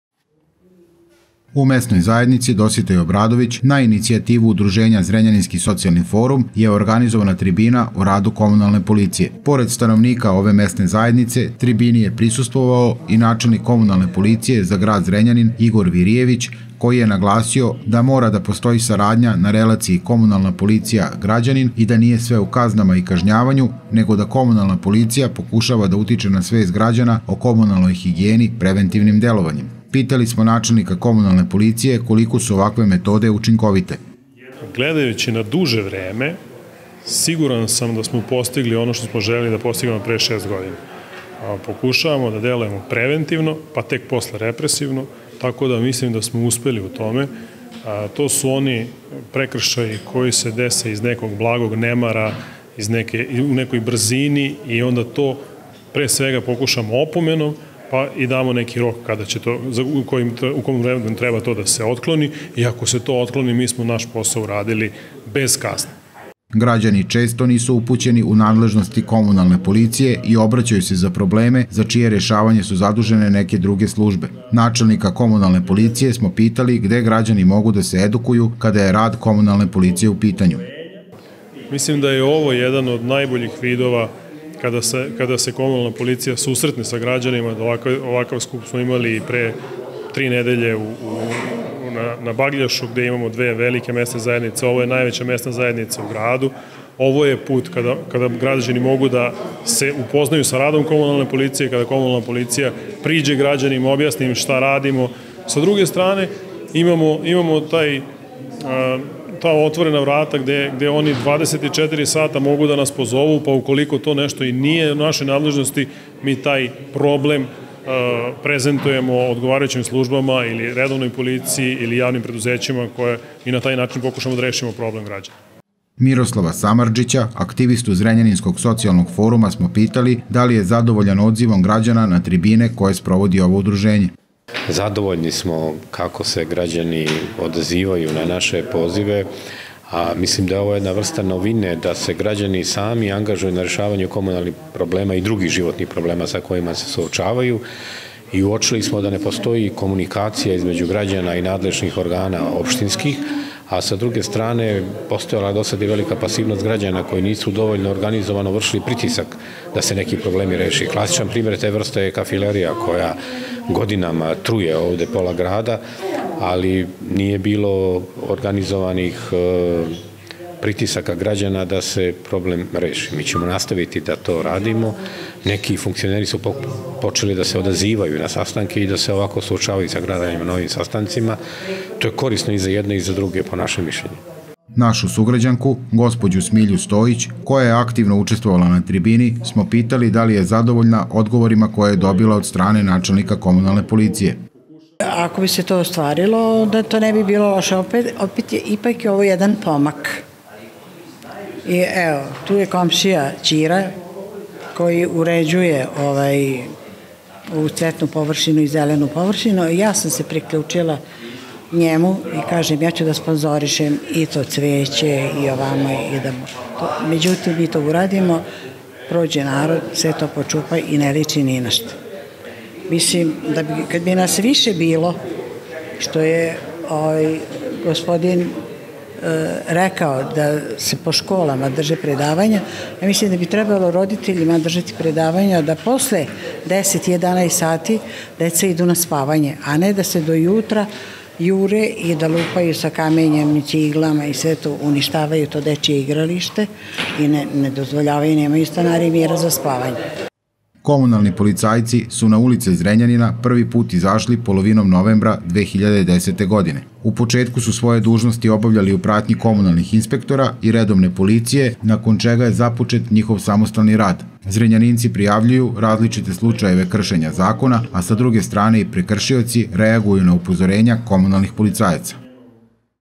U mesnoj zajednici “Dositej Obradović” je organizovana tribina o radu komunalne policije
U mesnoj zajednici “Dositej Obradović” na inicijativu udruženja Zrenjaninski socijalni forum, je organizovana tribina o radu komunalne policije.
Pored stanovnika ove mesne zajednice tribini je prisustvovao i načelnik komunalne policije za grad Zrenjanin Igor Virijević, koji je naglasio da mora da postoji saradnja na relaciji komunalna policija-građanin i da nije sve u kaznama i kažnjavanju, nego da komunalna policija pokušava da utiče na svest građana o komunalnoj higijeni preventivnim delovanjem.
Pitali smo načelnika komunalne policije koliko su ovakve metode učinkovite
Komunalni policajci su na ulice Zrenjanina prvi put izašli polovinom novembra 2010. godine. U početku su svoje dužnosti obavljali u pratnji komunalnih inspektora i redovne policije, nakon čega je započet i njihov samostalni rad. Zrenjaninci prijavljuju različite slučajeve kršenja zakona, a sa druge strane i prekršioci reaguju na upozorenja komunalnih policajaca